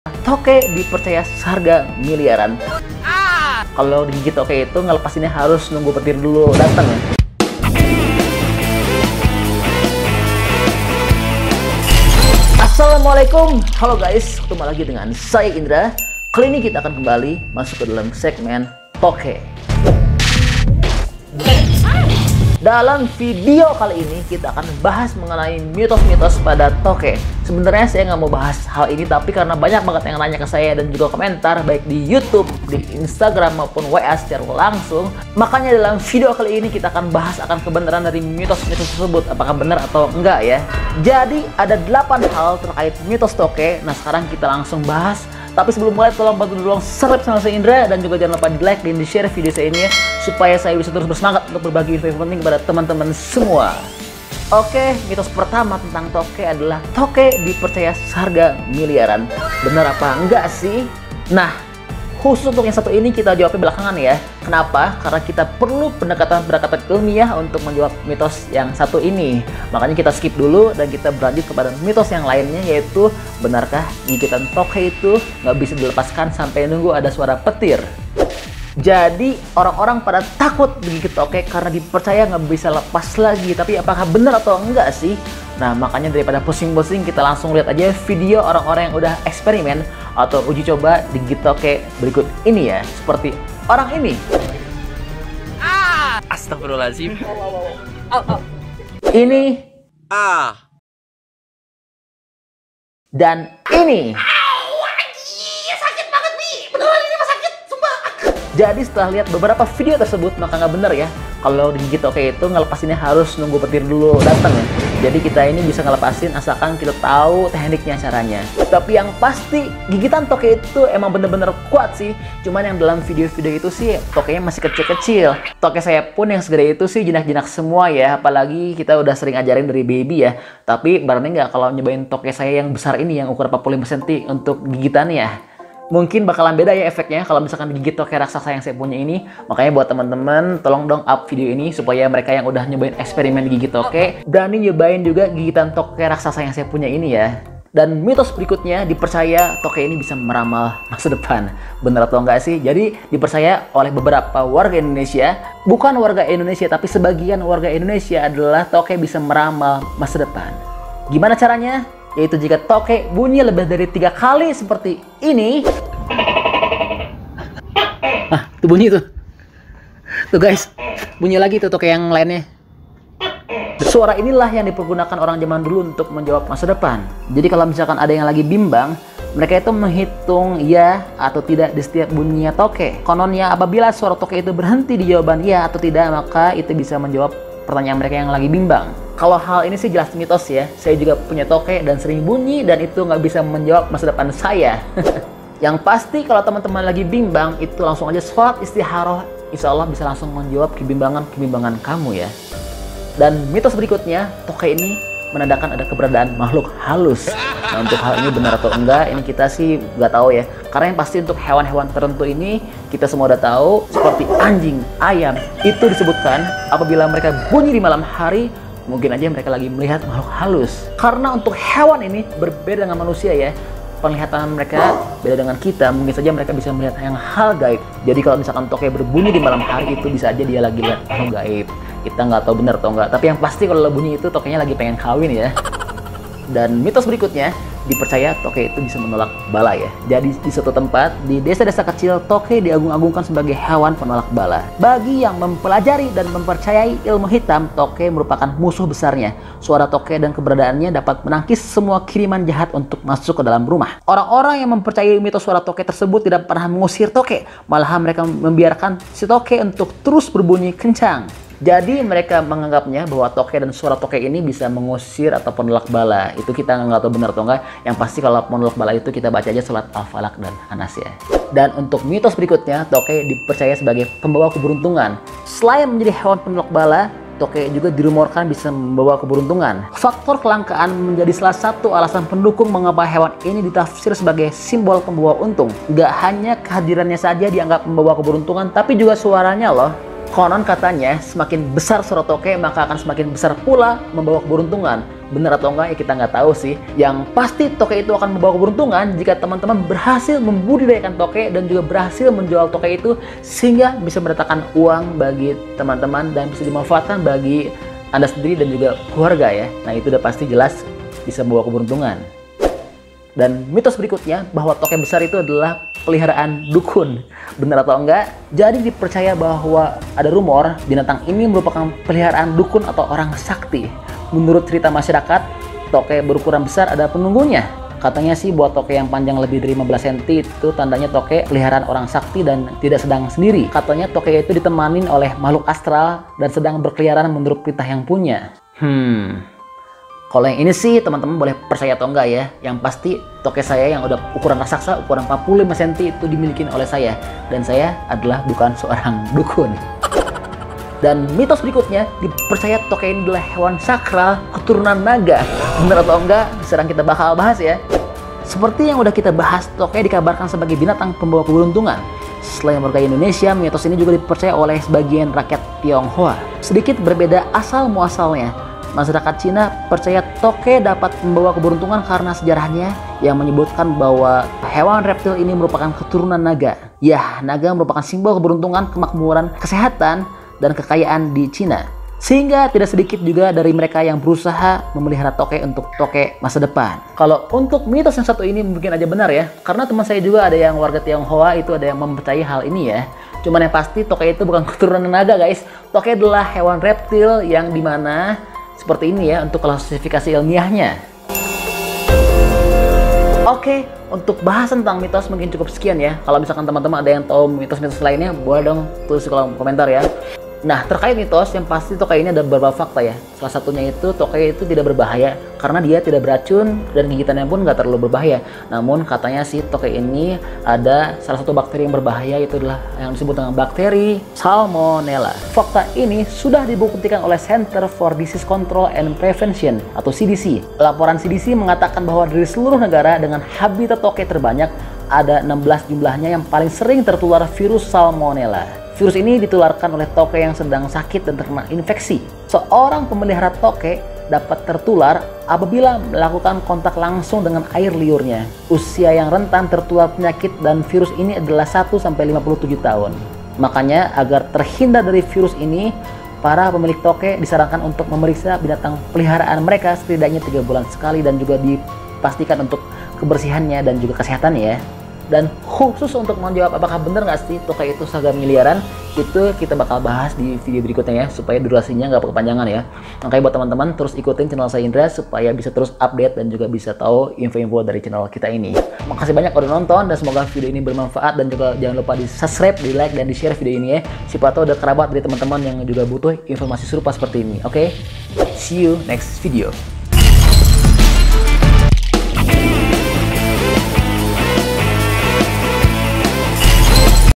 8 Mitos Tokek | Miliaran??
Video kali ini kami berbagi mitos pada tokek yang dipercaya oleh sebagian warga indonesia.

Ada beberapa mitos tokek, diantara:

1. Harga tokek miliaran
2. Gigitan tokek tidak akan bisa lepas hingga datang bunyi petir
3. Tokek bisa meramal masa depan
4. Tokek menandakan kedatangan makhluk halus
5. Tokek bisa menjadi alat untuk penolak bala
6. Tokek besar dipelihara dukun dan ada penunggunya
7. Tokek dapat membawa keberuntungan
8. Tokek merupakan makhluk astral

Benarkah mitos2 pada tokek tersebut?
Selengkapnya kita bahas dalam video ini, jangan di skip ya, supaya tidak ada informasi yang terlewat.

Terima kasih.
Selamat menonton.